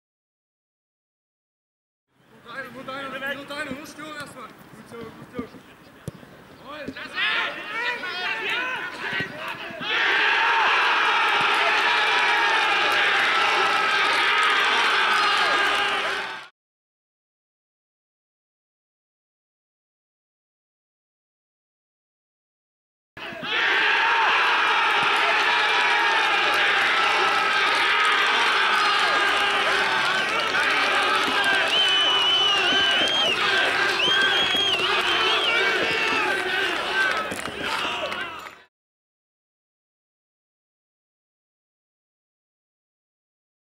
Tor: FV Blau-Weiß Zschachwitz 1:1 Dresdner SC 1898
In der 55. Minute gelingt Alexander Preißiger der Ausgleich. Vorlagengeber war Nikolaus Kuon